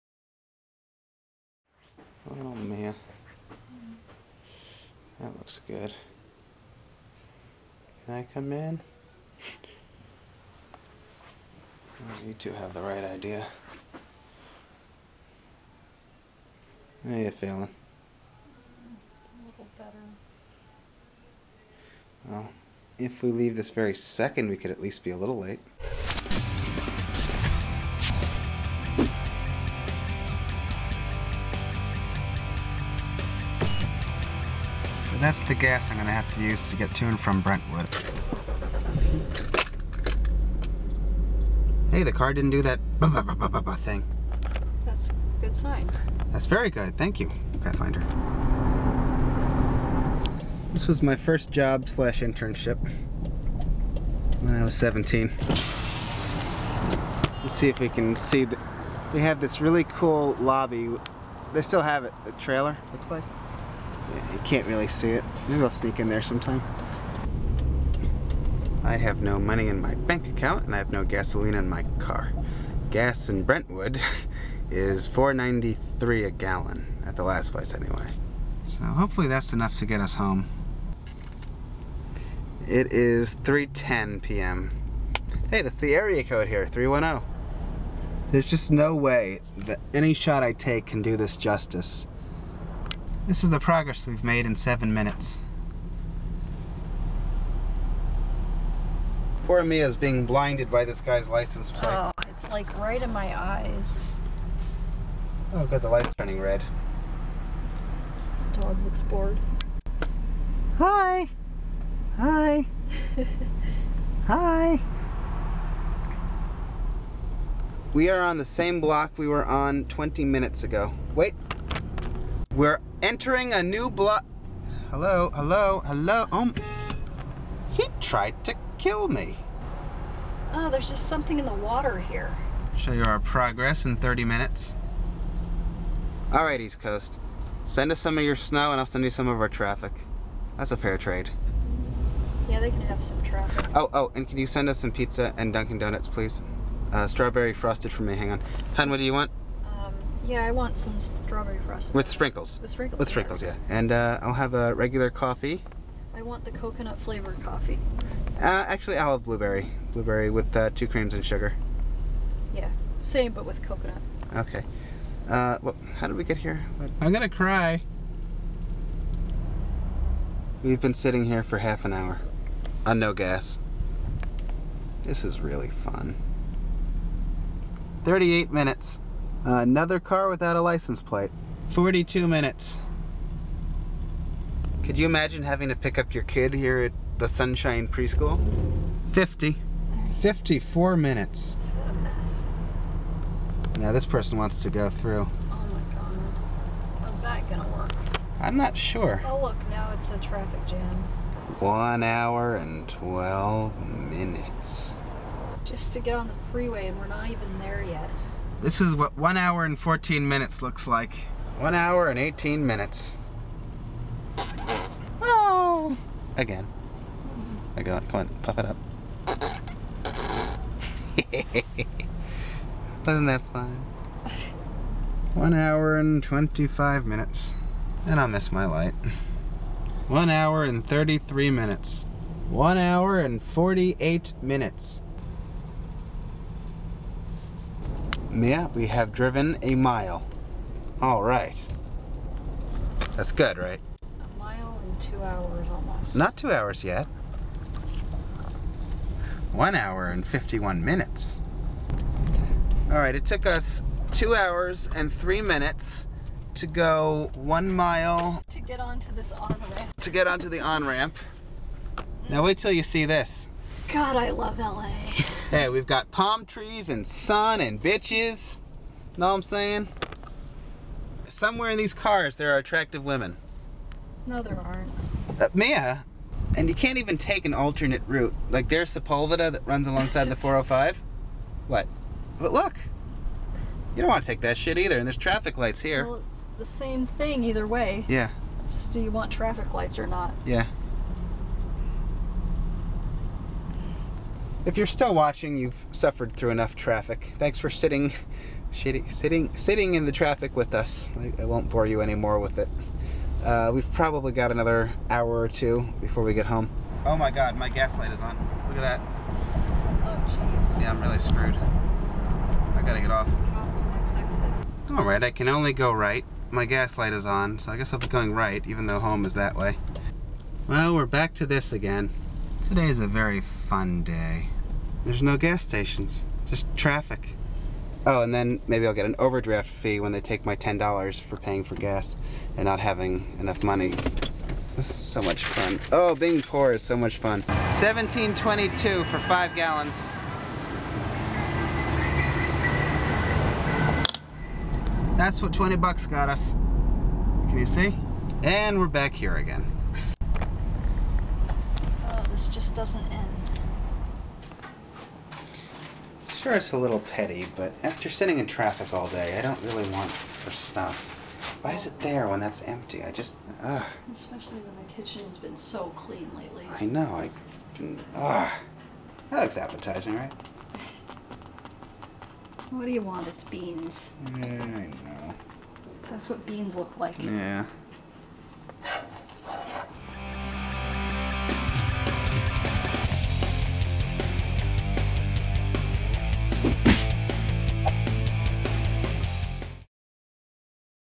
LA TRAFFIC SUCKS! (2.4.11 Vlog 58)
Mia and I met with a client today in Santa Monica. While I can't talk about or show anything regarding client work, you can have a look at what our commute looks like. This isn't a freak thing, this isn't because of construction, it's been this way as long as we've lived in LA (well, long before we got there!). Between about noon and 6pm, it could take over an hour just to get on the 405 North. No, public transportation is not an option.